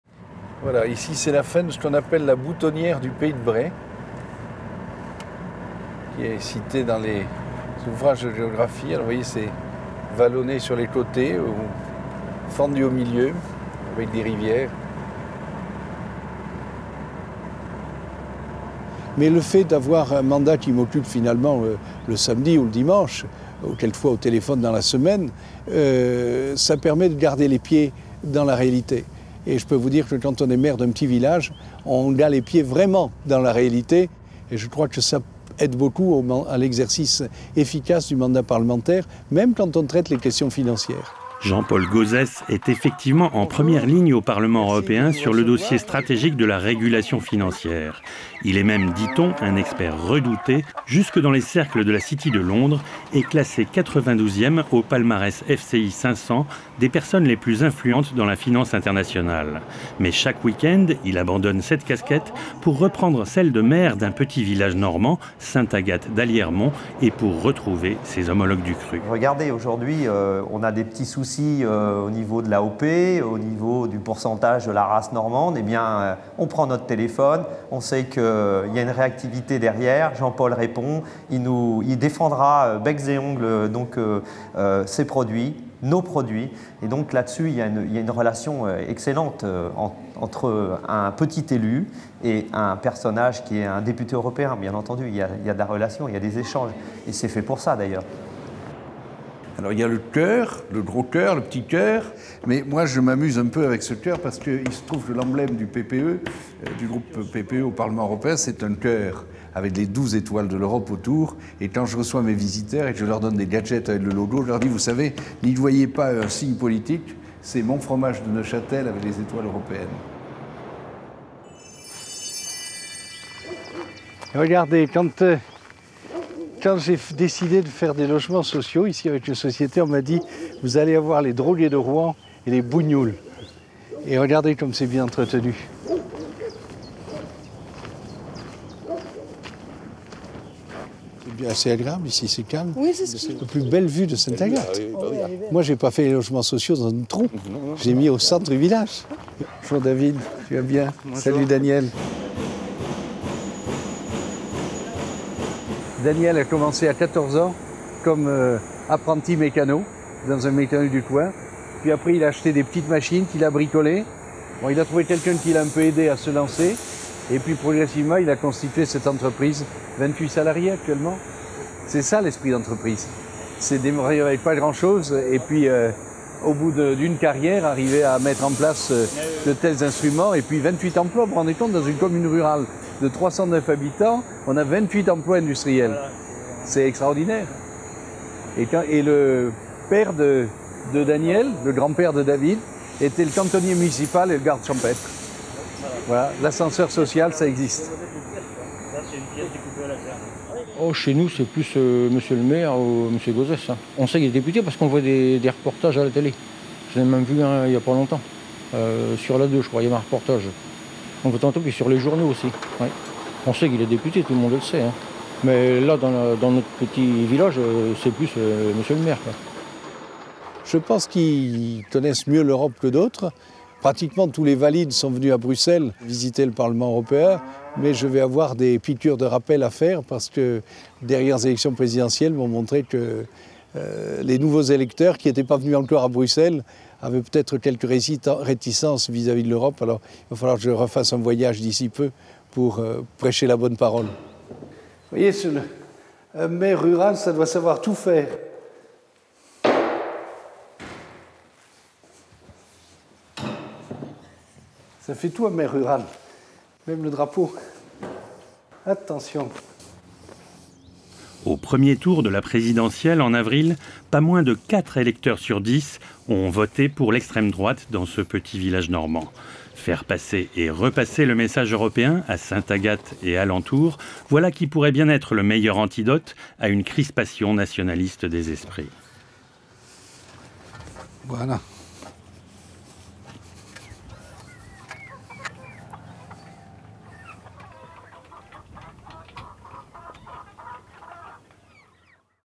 Jean-Paul Gauzès, élu normand
Au Parlement européen, Jean-Paul Gauzès est en première ligne sur le dossier de la régulation financière. Mais chaque week-end, il abandonne cette casquette pour celle de maire d'un petit village normand, Sainte-Agathe d'Aliermont. *Réalisé pour EuroparlTV